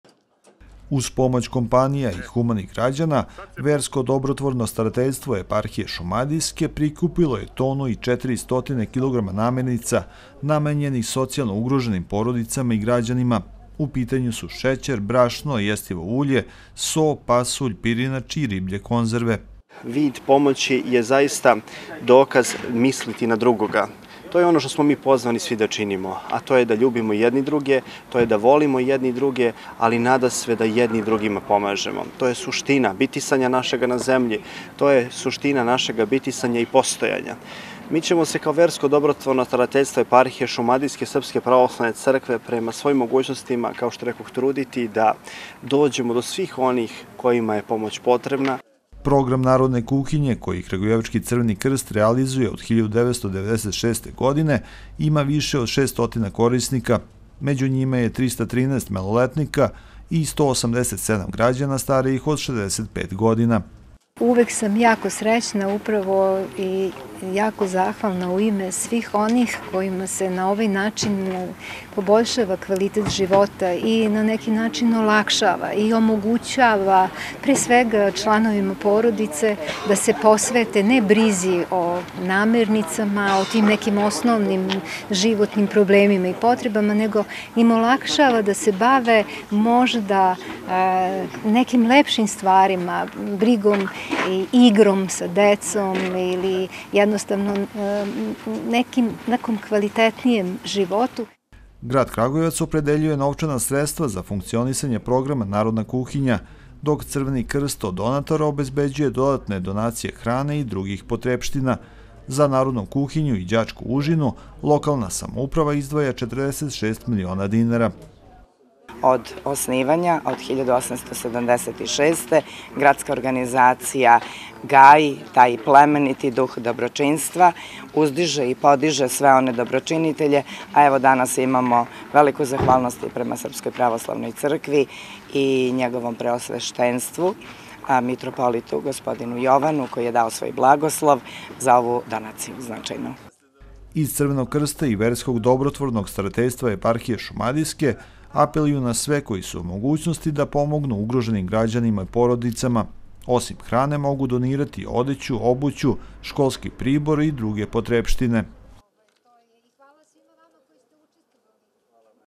Eparhija šumadijska donirala tonu i po hrane Crvenom krstu
Versko dobrotvorno starateljstvo Eparhije šumadijske prikupilo je i doniralo skoro tonu i po životnih namirnica gradskoj organizaciji Crvenog krsta.